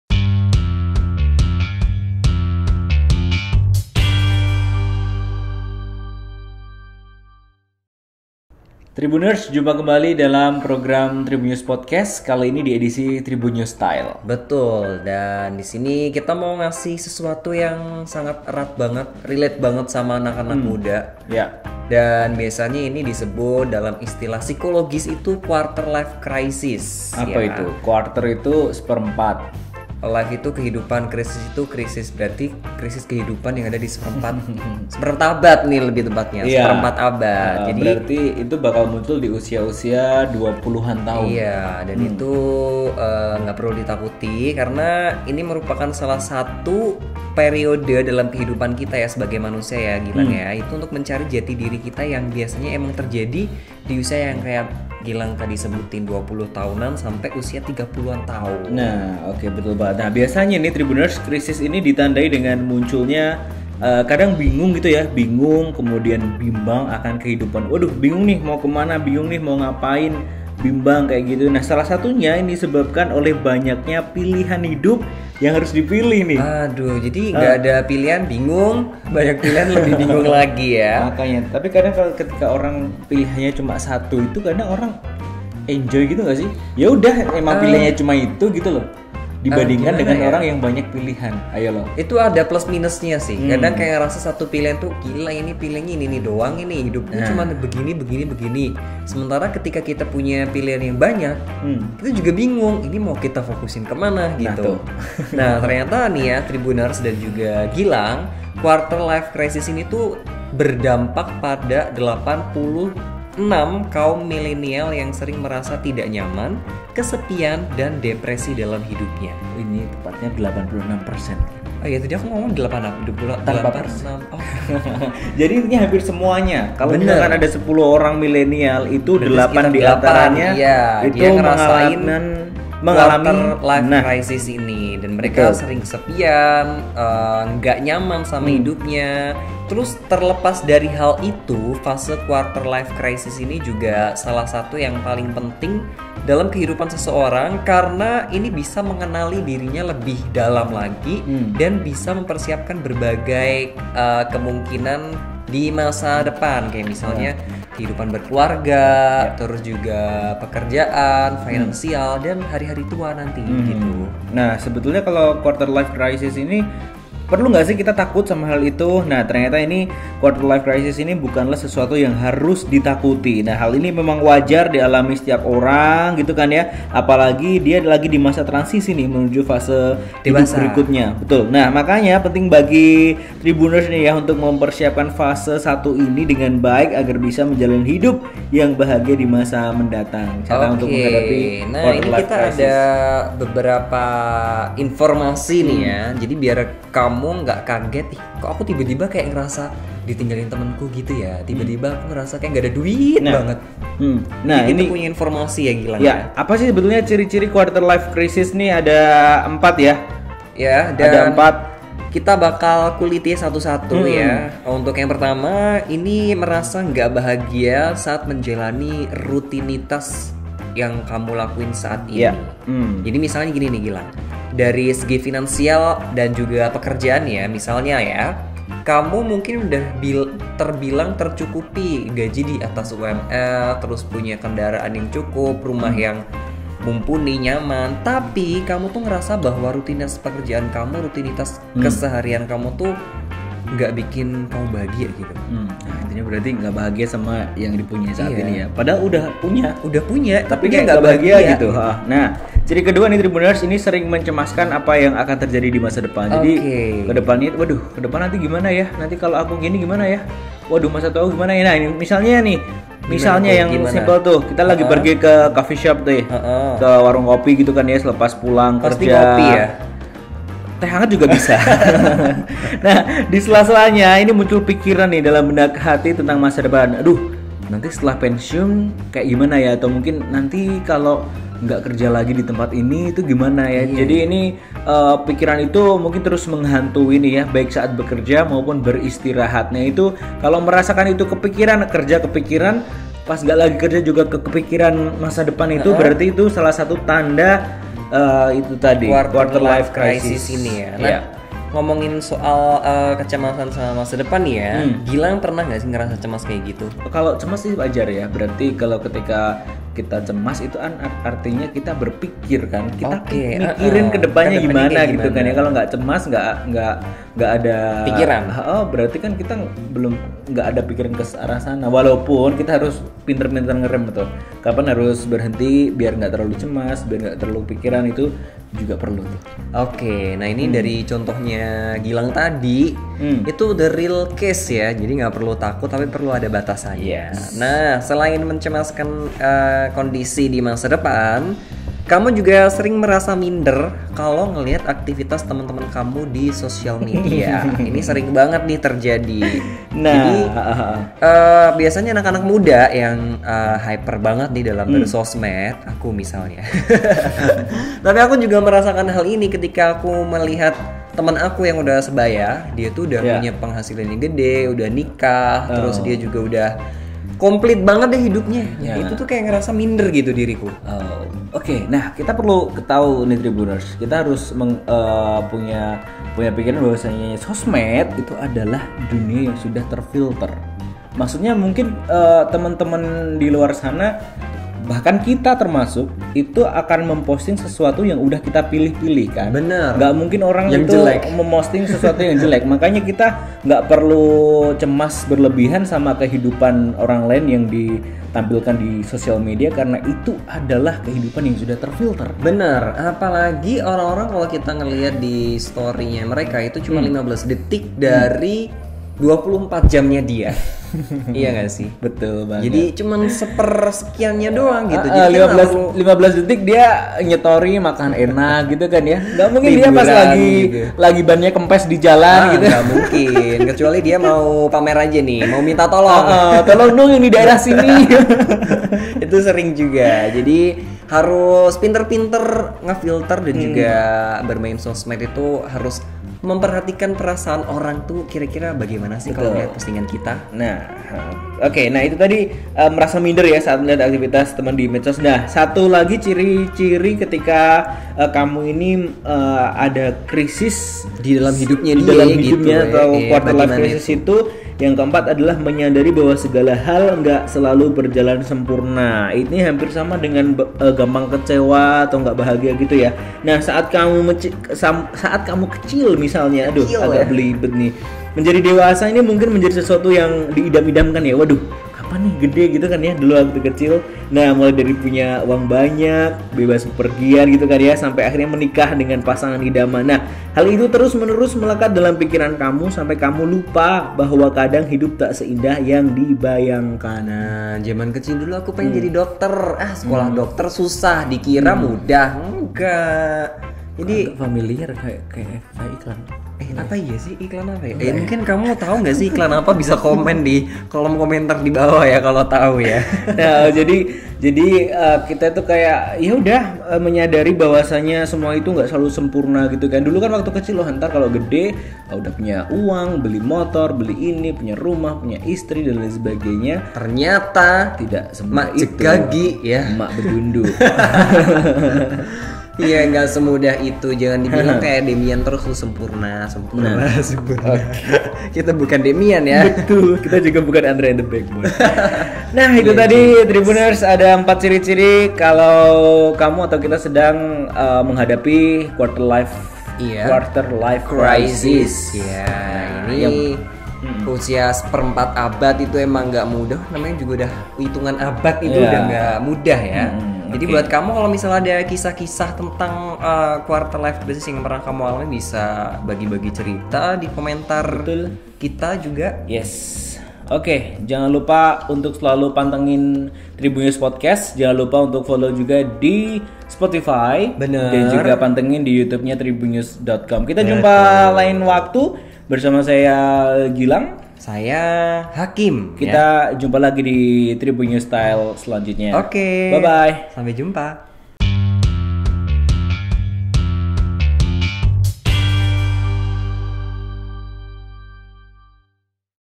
Krisis Kehidupan di Usia 25 Tahun, Pernah Merasakan Galau Jenis Apa Saja?
TRIBUNNEWS.COM - Usia 25 tahun nampaknya menjadi tolok ukur dalam kehidupan, karena usia itu kita dihadapkan dengan banyak pilihan. Kalau tidak bisa perlahan-lahan dalam menapaki hidup di usia itu, efeknya bisa terasa di masa depan.